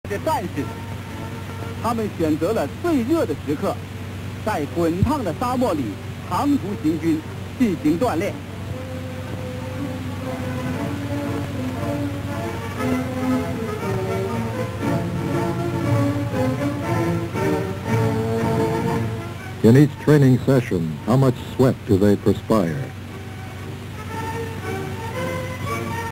Sweating In Chemical Protective Clothing 1966
When responding to a chemical spill or other hazardous cleanup operation, first responders and cleanup workers must frequently wear a hazardous materials suit. These suits protect the first responder from chemical exposure.  Unfortunately, they quickly get very hot and humid. Given the fact that a first responder can be in the suit from 30-60 min, overheating is not just a source of discomfort, but is a real hazard to the health of the first responder. The heat/humidity buildup in the suit severely limits the time that can be spent in the suit without risking heat exhaustion.  For more on heat stress and its prevention, go to the NIOSH Heat Stress . This was clipped from a 1966 film from the People's Republic of China, with English narration.  The film shows the emergence of China as a nuclear power and describes the efforts of the Chinese in preparation and conduction of atomic and thermo-nuclear tests in the mid-1960s.